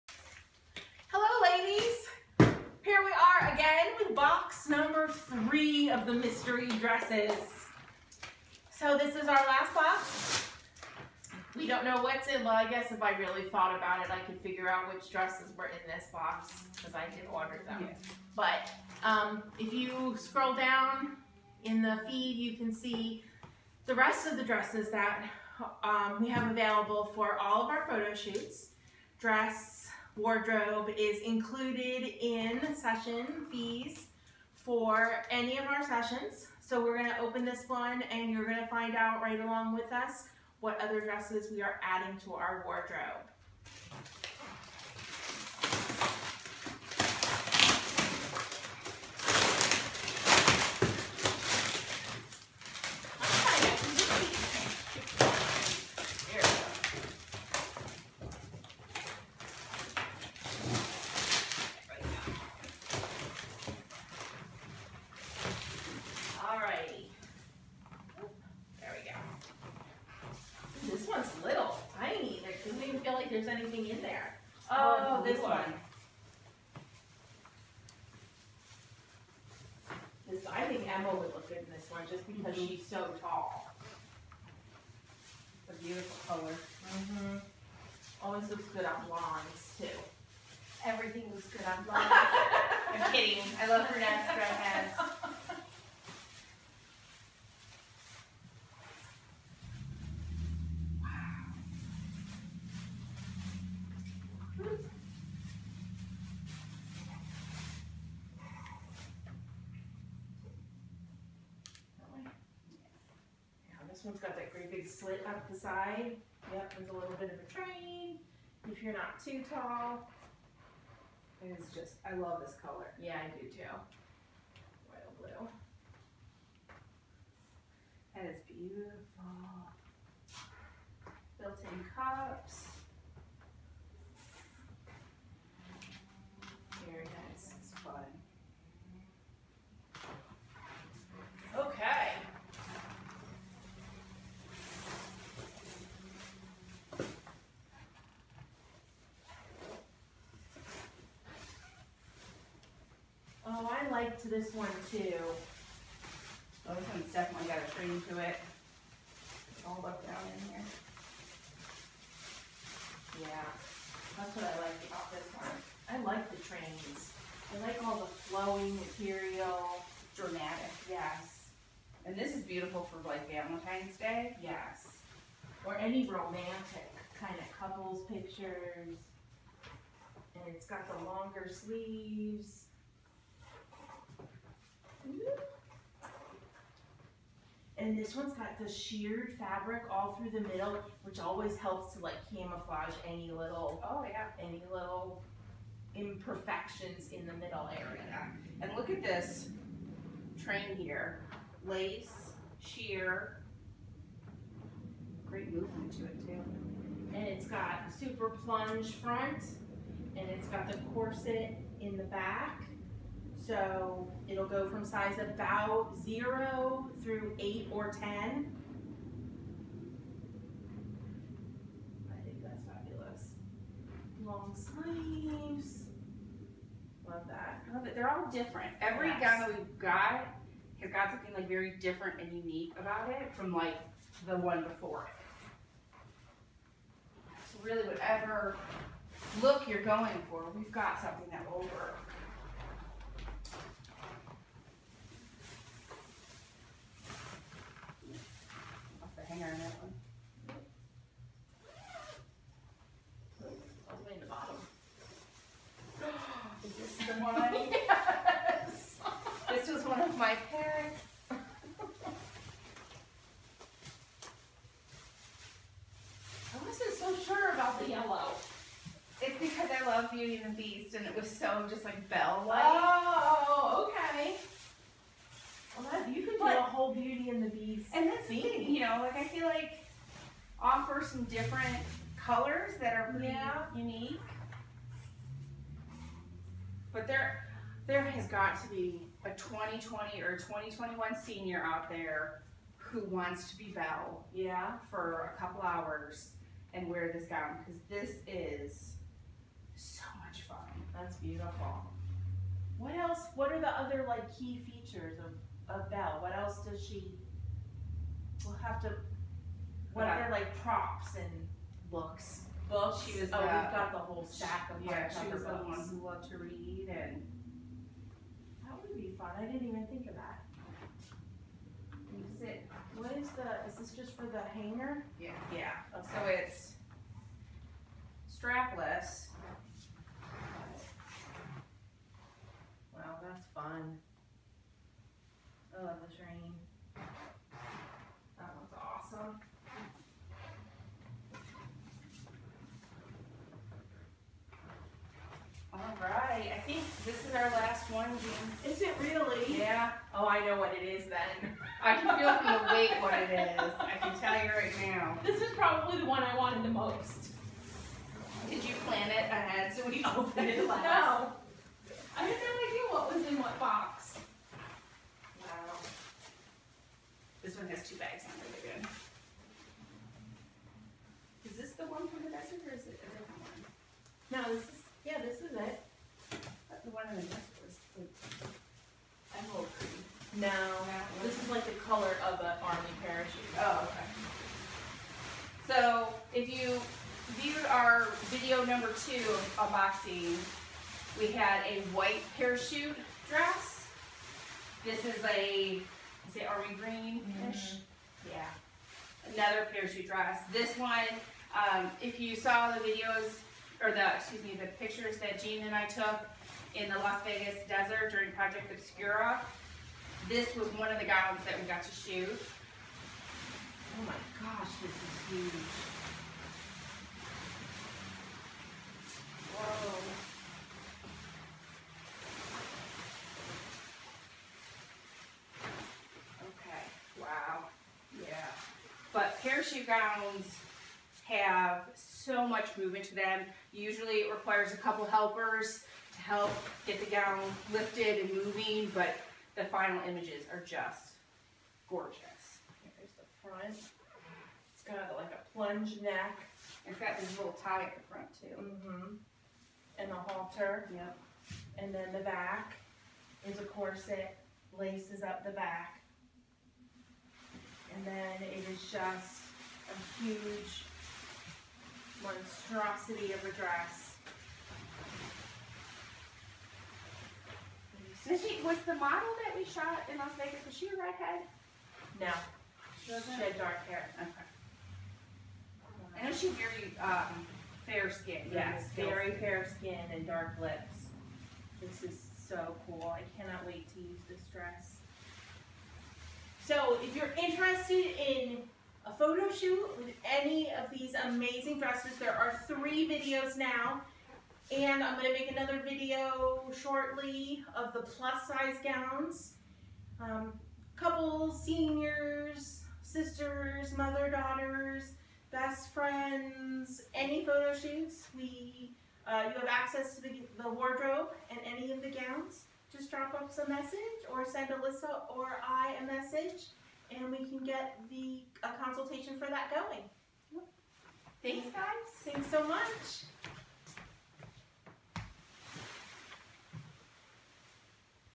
Extreme Luxury Gowns for Portraits at Pearlescent Photography - Box 3
Pearlescent Photo Gowns available for use for Senior Portrait, Couple, Engagement, Beauty, Glamour, Mother/Daughter, Generation and other luxury photo sessions.